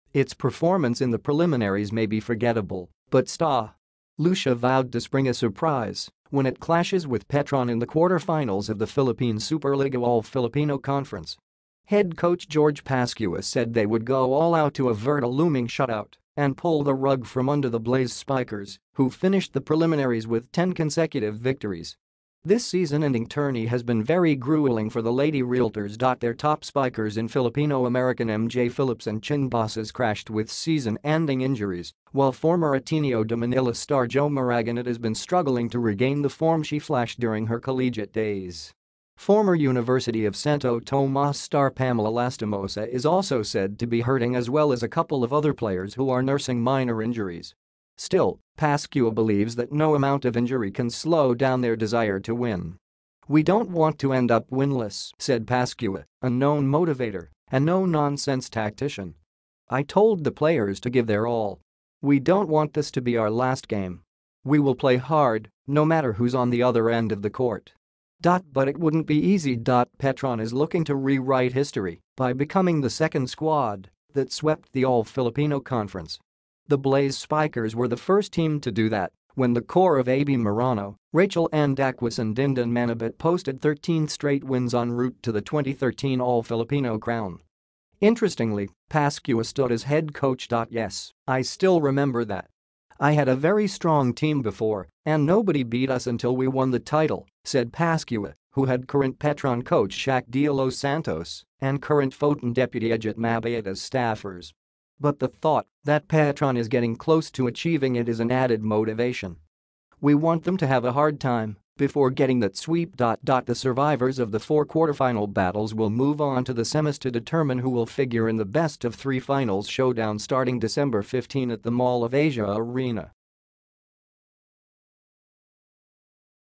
Sta. Lucia eyes massive upset over Petron in PSL quarters
Its performance in the preliminaries may be forgettable, but Sta. Lucia vowed to spring a surprise when it clashes with Petron in the quarterfinals of the Philippine Superliga All-Filipino Conference.

Head coach George Pascua said they would go all out to avert a looming shutout and pull the rug from under the Blaze Spikers, who finished the preliminaries with 10 consecutive victories.

This season-ending tourney has been very grueling for the Lady Realtors.

Their top spikers in Filipino-American MJ Philips and Chin Basas crashed with season-ending injuries while former Ateneo de Manila star Jho Maraguinot has been struggling to regain the form she flashed during her collegiate days.

Former University of Santo Tomas star Pamela Lastimosa is also said to be hurting as well as a couple of other players who are nursing minor injuries.

Still, Pascua believes that no amount of injury can slow down their desire to win.

“We don’t want to end up winless,” said Pascua, a known motivator and no non-sense tactician.

“I told the players to give their all. We don’t want this to be our last game. We will play hard no matter who’s on the other end of the court.”

But it wouldn’t be easy.

Petron is looking to rewrite history by becoming the second squad that swept the All-Filipino Conference.

The Blaze Spikers were the first team to do that when the core of Aby Marano, Rachel Anne Daquis and Dindin Manabat posted 13 straight wins en route to the 2013 All-Filipino crown.

Interestingly, Pascua stood as head coach.

“Yes, I still remember that. I had a very strong team before and nobody beat us until we won the title,” said Pascua, who had current Petron coach Shaq Delos Santos and current Foton deputy Edjett Mabbayad as staffers.

“But the thought that Petron is getting close to achieving it is an added motivation. We want them to have a hard time before getting that sweep.”

The survivors of the four quarterfinal battles will move on to the semis to determine who will figure in the best-of-three finals showdown starting December 15 at the Mall of Asia Arena.